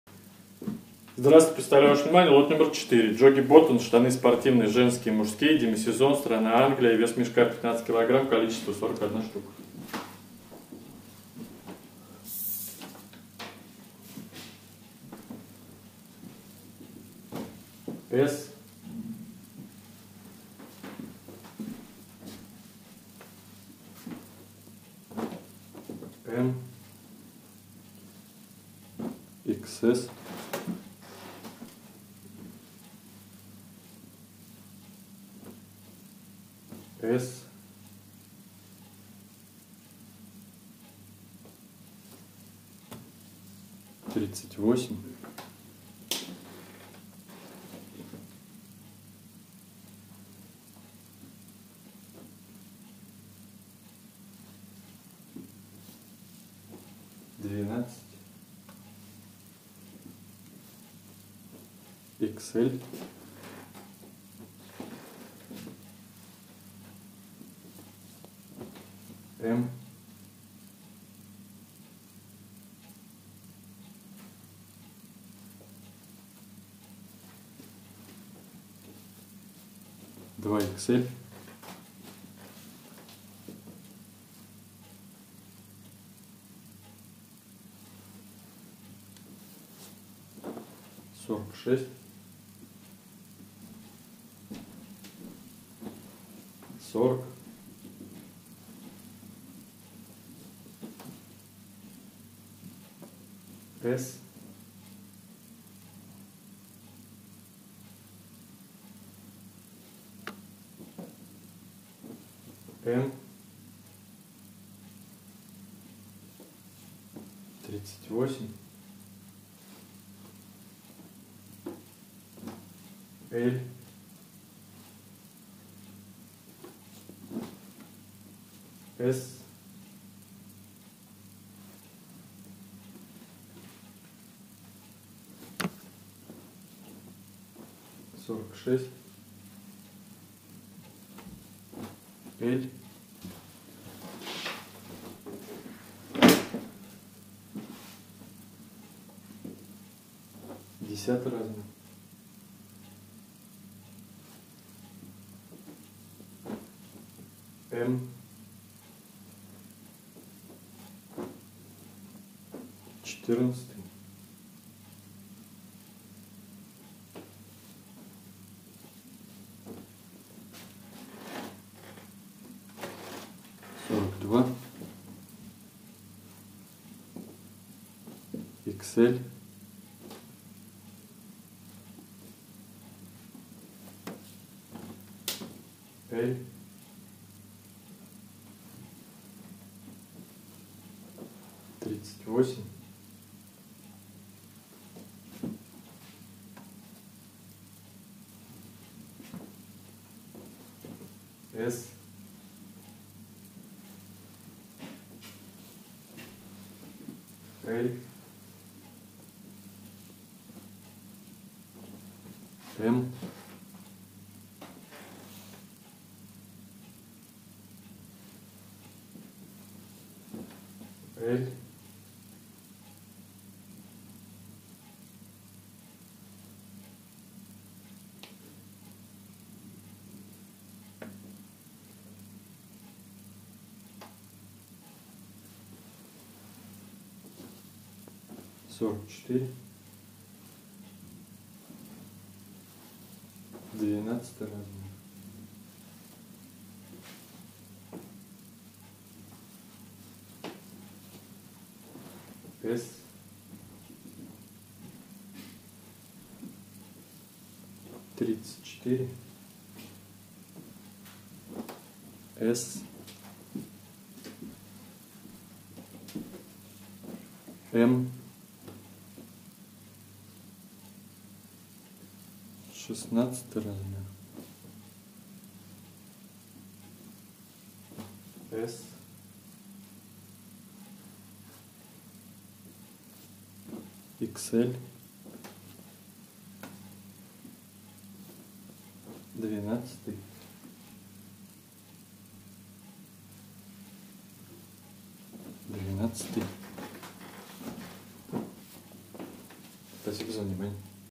Jogging Botton mix 4,Цена за кг: 6.21 евро, секонд хенд оптом
Jogging Botton mix (лот № 4)
Спортивные штаны (секонд хенд)
Страна: Англия
Сезон: Демисезон
Цена за кг.: 6.21 евро
Цена за мешок: 93.15 евро
Цена за ед.: 2.27 евро